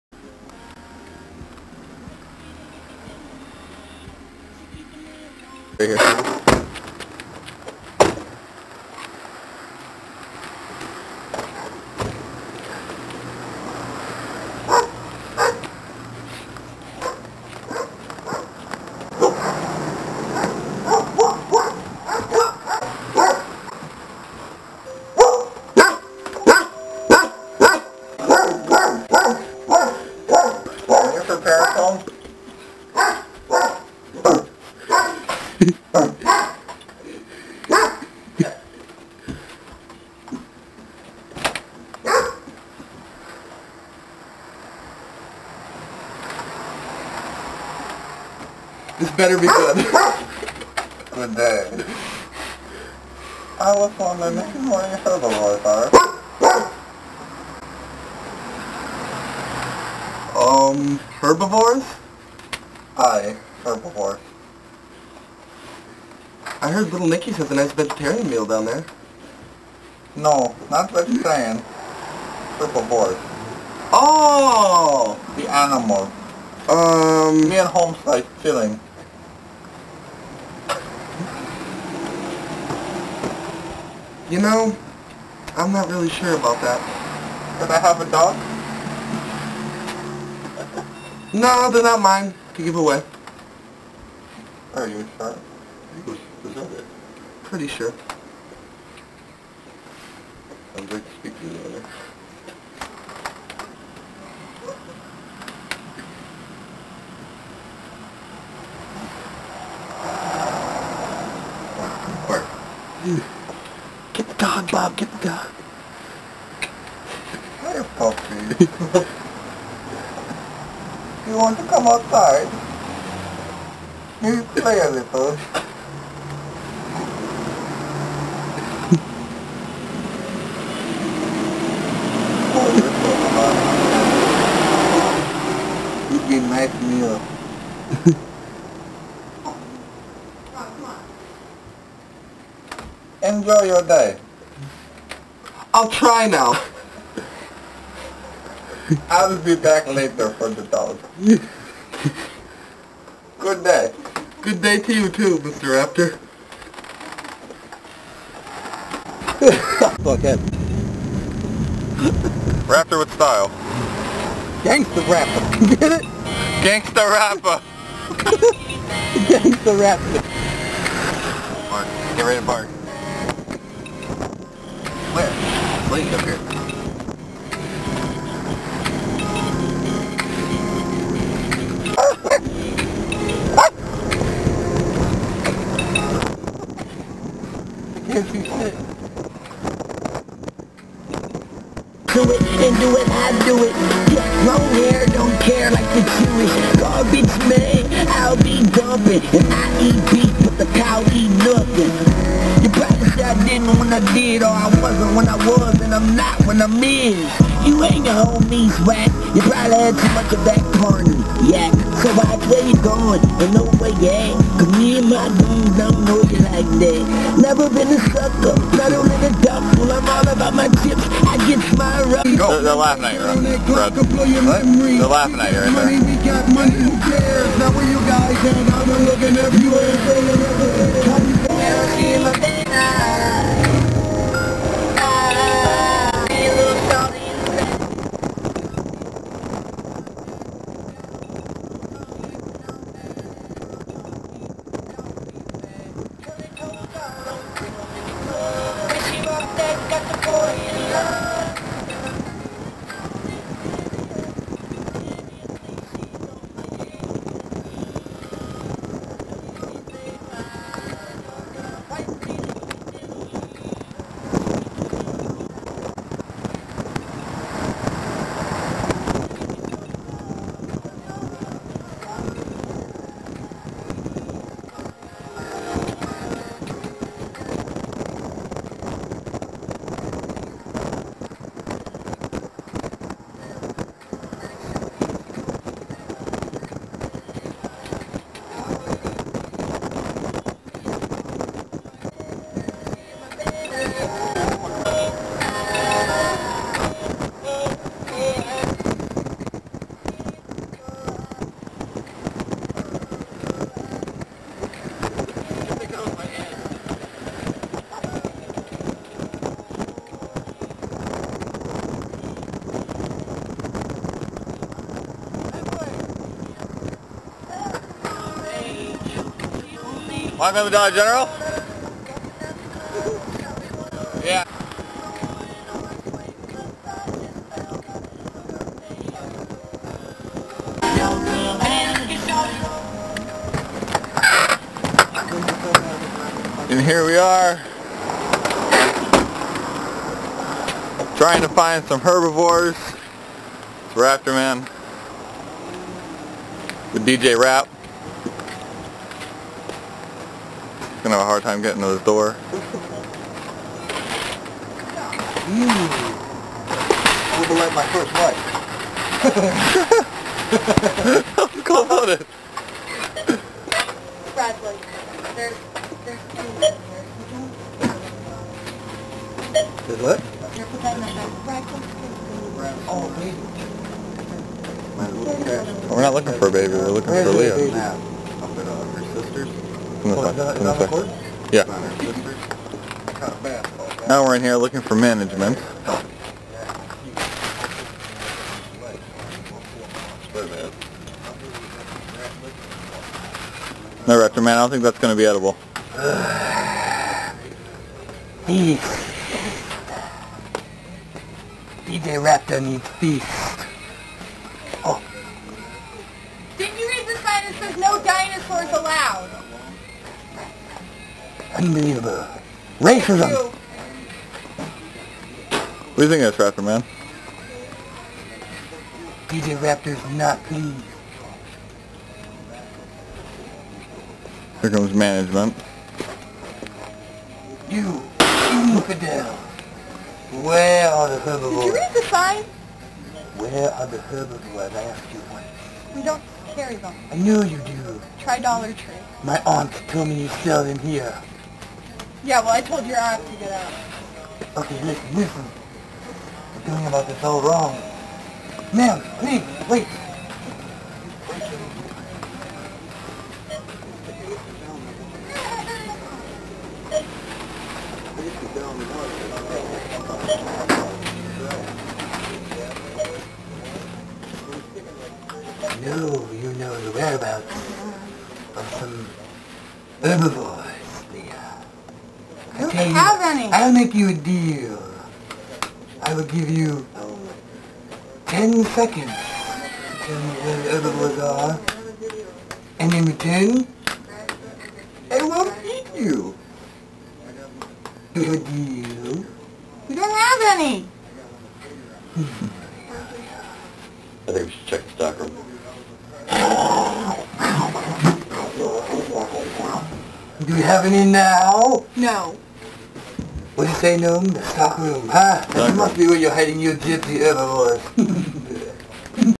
Raptor Video
This is just a video of my friends and I being stupid around town. I can't seem to get rid of those grey lines but it's only a few videos I have with Ronnie in them.